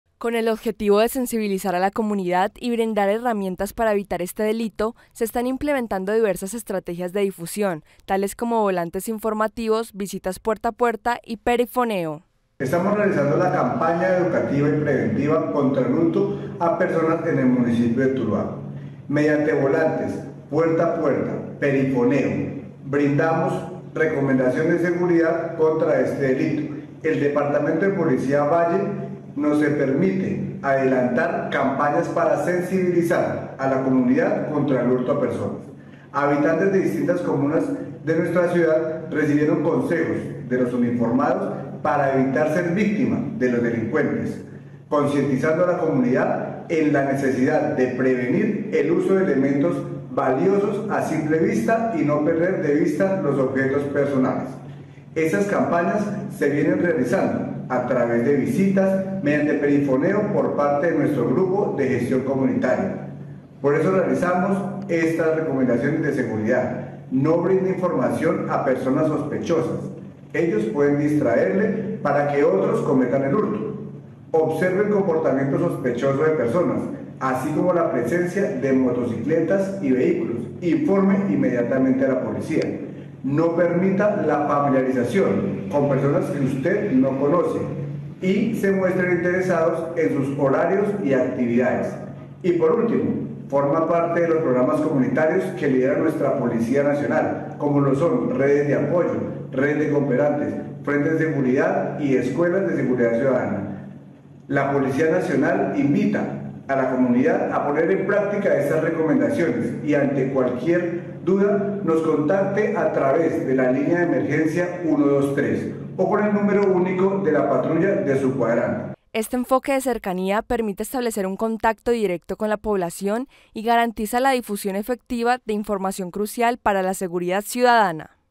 Campaña Educativa y Preventiva contra el hurto a personas en el municipio de Tuluá.
Con el objetivo de fortalecer la seguridad y conciencia ciudadana en el municipio de Tuluá, El Departamento de Policía Valle, ha puesto en marcha una campaña educativa y preventiva contra el hurto a personas.